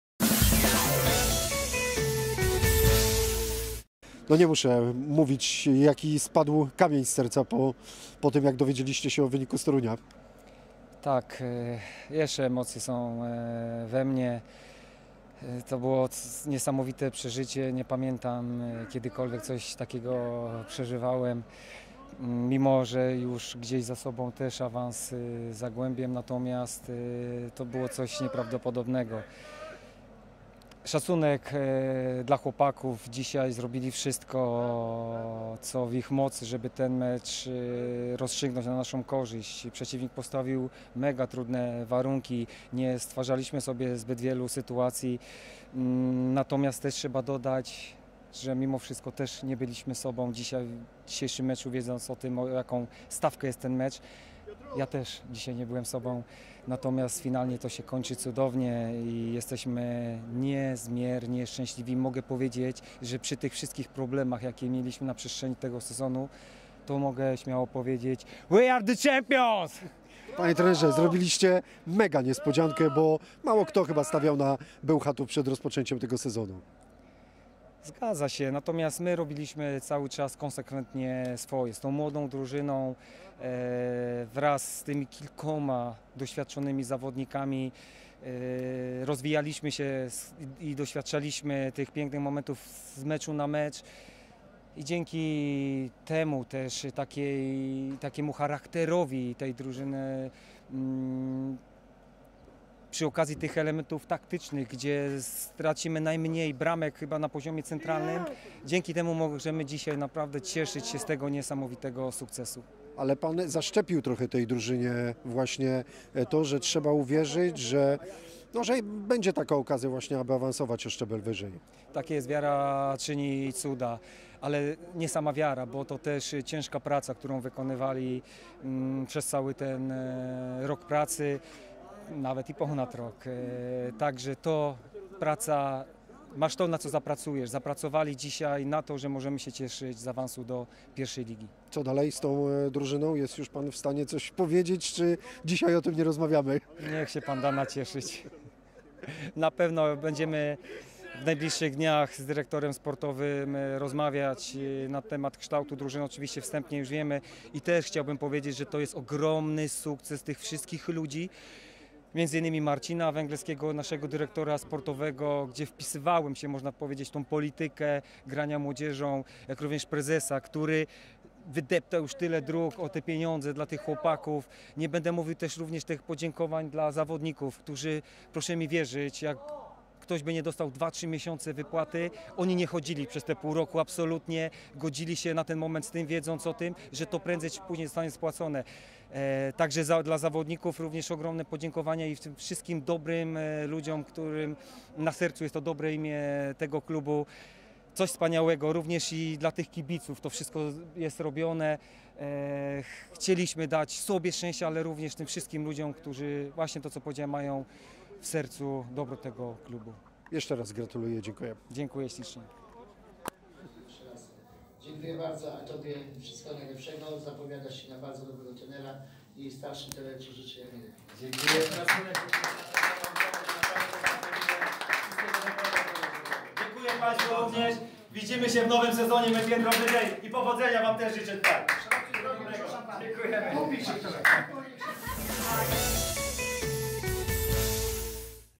Trener Artur Derbin po awansie do I ligi. Błękitni Stargard - PGE GKS Bełchatów 0:0
Piłkarze PGE GKS Bełchatów zremisowali na wyjeździe z Błękitnymi Stargard 0:0, ale wywalczyli awans do I ligi. Wielkiej radości po spotkaniu nie krył trener Artur Derbin, który spontaniczny wyraz radości dał podczas rozmowy z Radiem Łódź. Po konferencji otrzymał też osobiste gratulacje od doświadczonego szkoleniowca Błękitnych - Adama Topolskiego. (19 maja 2019 r.)